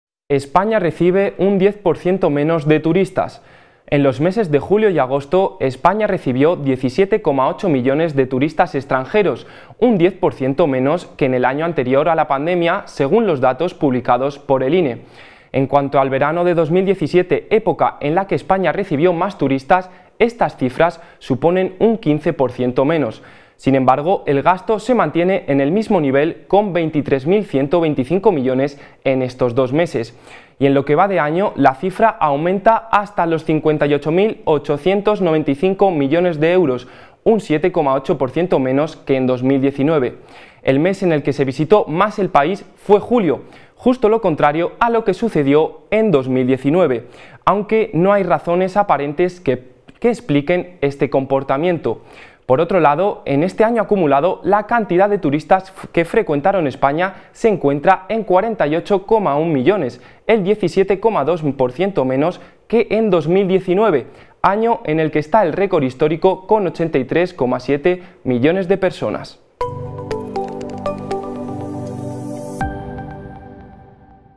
España recibe un 10% menos de turistas
En los meses de julio y agosto España recibió 17´8 millones de turistas extranjeros, un 10% menos que en el año anterior a la pandemia según los datos publicados por el INE. En cuanto al verano de 2017, época en la que España recibió más turistas estas cifras suponen un 15% menos. Sin embargo, el gasto se mantiene en el mismo nivel, con 23.125 millones en estos dos meses. Y en lo que va de año la cifra aumenta hasta los 58.895 millones de euros, un 7´8% menos que en 2019. El mes en el que se visitó más el país fue julio, justo lo contrario a lo que sucedió en 2019, aunque no hay razones aparentes que expliquen este comportamiento. Por otro lado, en este año acumulado, la cantidad de turistas que frecuentaron España se encuentra en 48 ́1 millones, el 17 ́2% menos que en 2019, año en el que está el récord histórico con 83 ́7 millones de personas.